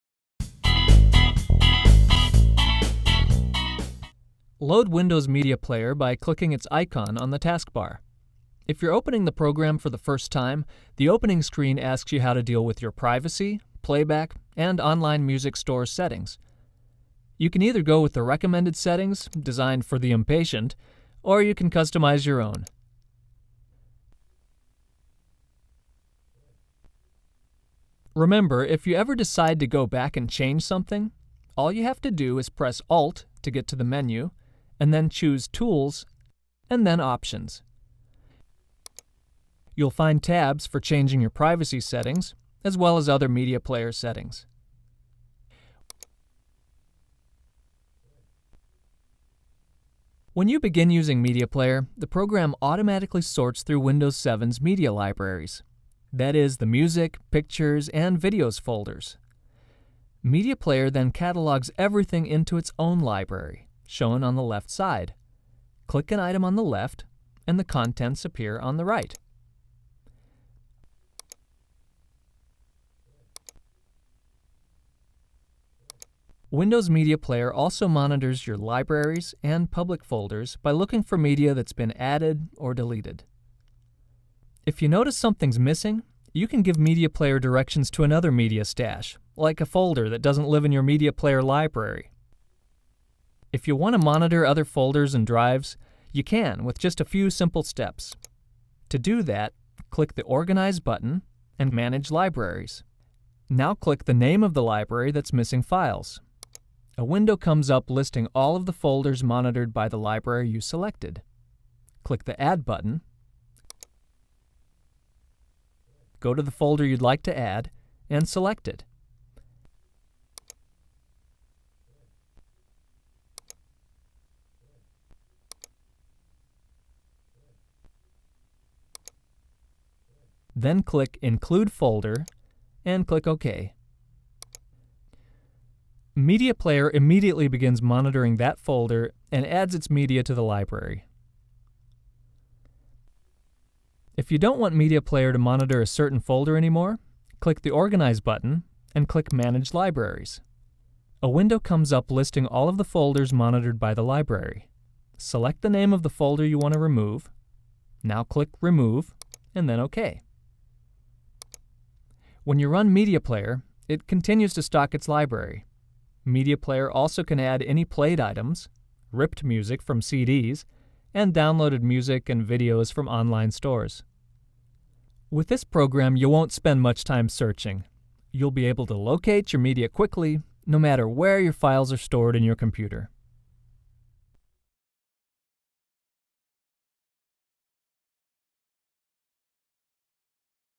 How to Tell Windows Media Player 12 Where Your Media Files Are Kept For Dummies
In Windows 7, Windows Media Player 12 automatically looks for movies, music, and images in particular places and compiles libraries of your media. If you don't keep your media in one of those places, you need to tell Windows Media Player where else it should look. This video shows you how.